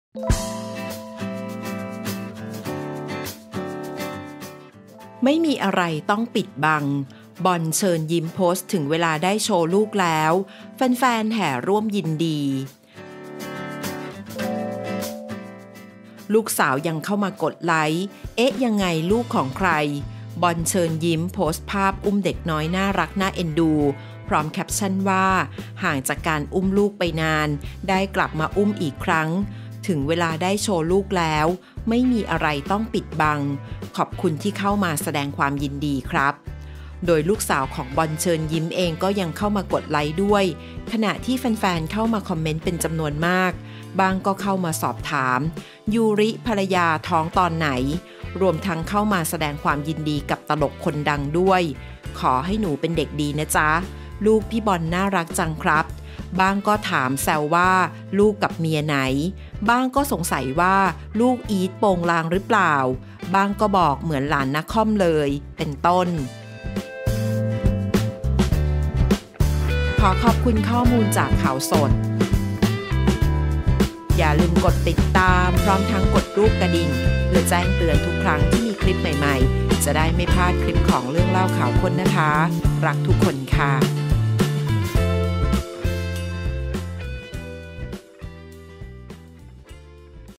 ไม่มีอะไรต้องปิดบัง! บอล เชิญยิ้ม โพสต์ถึงเวลาได้โชว์ลูกแล้ว แฟนๆร่วมยินดี
ลูกสาวยังเข้ามากดไลก์ เอ๊ะยังไงลูกใคร บอล เชิญยิ้ม โพสต์ภาพอุ้มเด็กน้อยน่าเอ็นดู พร้อมแคปชั่นว่า “ห่างจากการอุ้มลูกไปนาน ได้กลับมาอุ้มอีกครั้ง ถึงเวลาได้โชว์ลูกแล้ว ไม่มีอะไรต้องปิดบัง ขอบคุณที่เข้ามาแสดงความยินดีครับ”